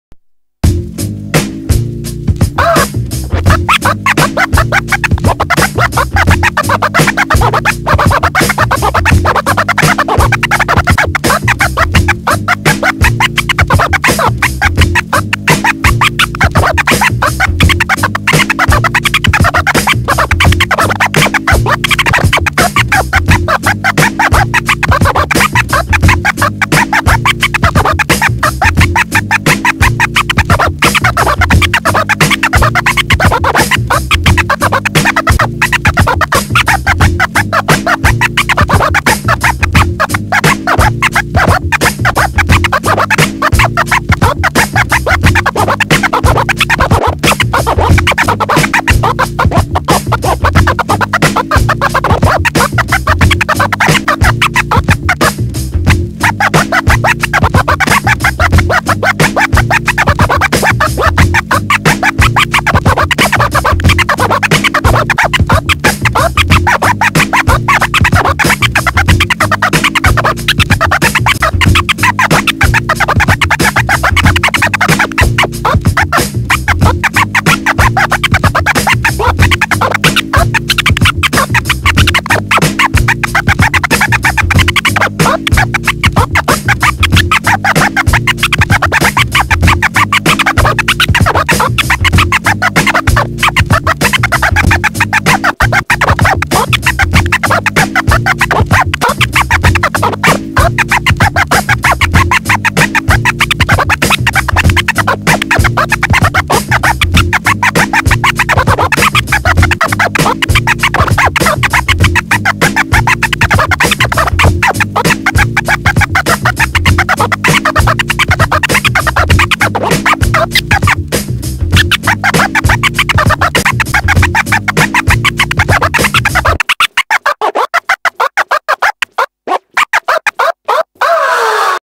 Chirp Scratch Practice
Regular to triplet time chirps over a speed booster rising from 85-100bpm